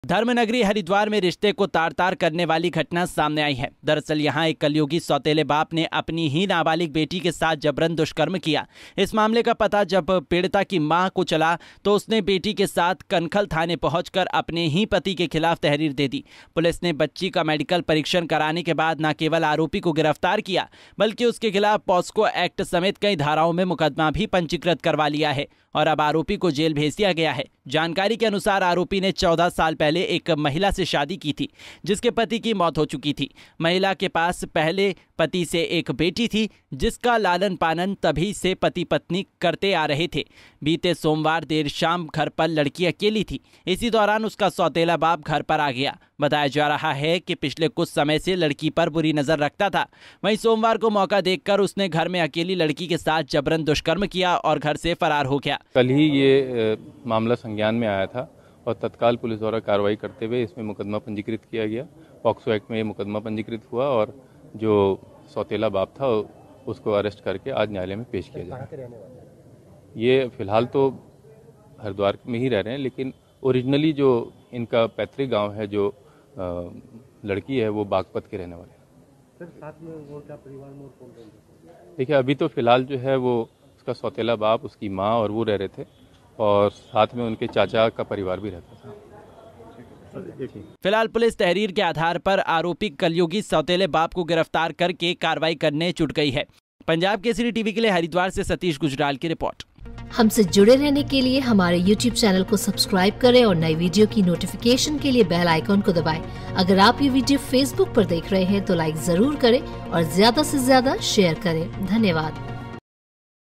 Haridwar में पिता ने रिश्ते को किया शर्मसार,पिता ने किया नाबालिक बेटी से दुष्कर्म
धर्म नगरी हरिद्वार(Haridwar) में एक कलयुगी सौतेले बाप ने अपनी ही नाबालिग बेटी के साथ जबरन दुष्कर्म किया...पुलिस ने तहरीर मिलने के बाद आरोपी पिता को गिरफ्तार किया...